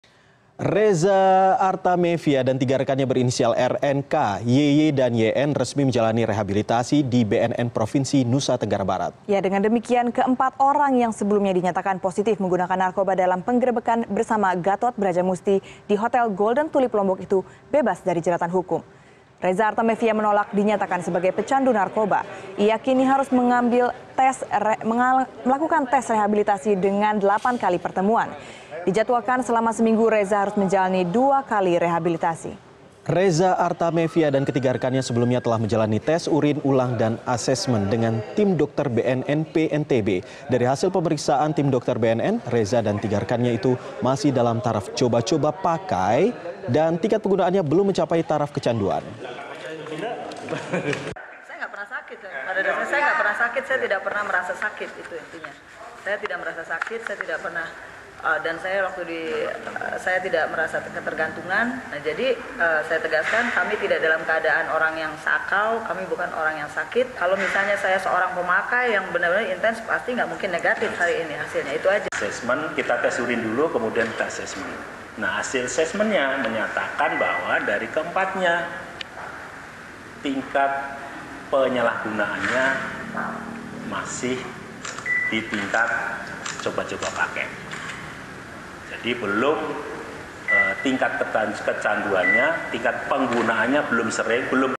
Reza Artamevia Buka Suara terkait Penangkapannya
Reza Artamevia bersama 3 rekannya resmi menjalani rehabilitasi di BNN Provinsi Nusa Tenggara Barat. Dengan demikian, 4 orang yang sebelumnya dinyatakan positif narkoba dalam penangkapan bersama Gatot Brajamusti bebas dari jeratan hukum.